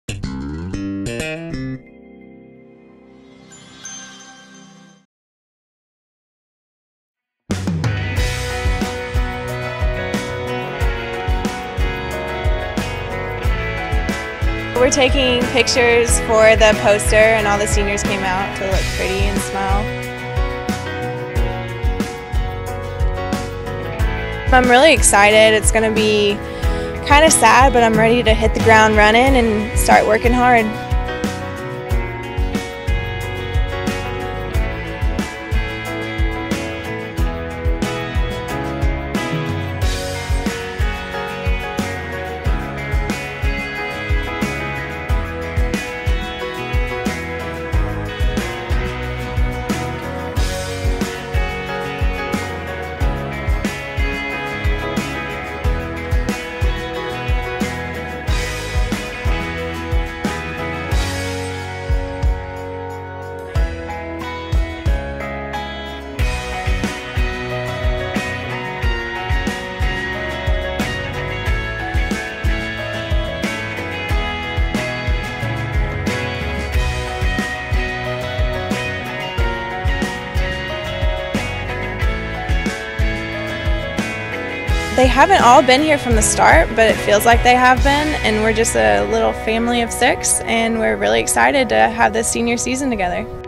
Tennessee Soccer: 2013 Senior Photo Shoot (7/23)
Tennessee soccer seniors Tori Bailey, Caroline Brown, Caroline Capocaccia, CC Cobb, Katie Lenz and Jessica Rolfs took photos on July 23 for the 2013 UT Soccer schedule poster.